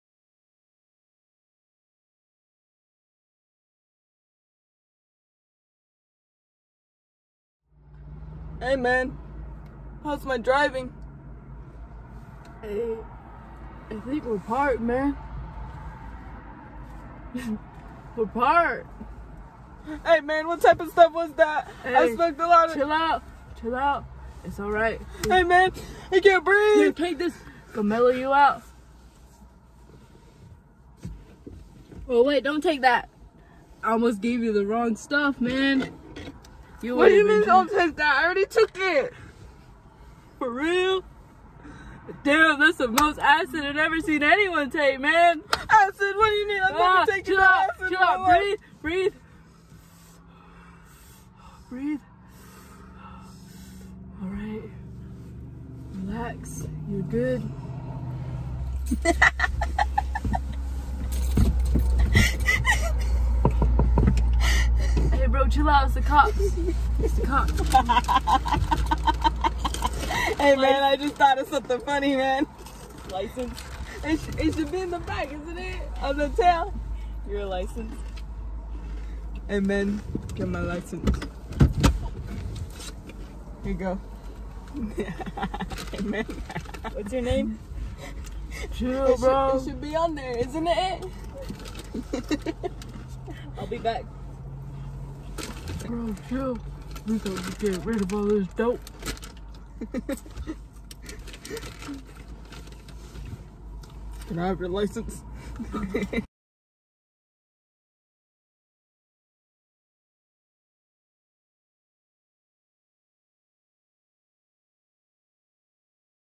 Remake That Shot
Remake of Cheech and Chong, driving scene.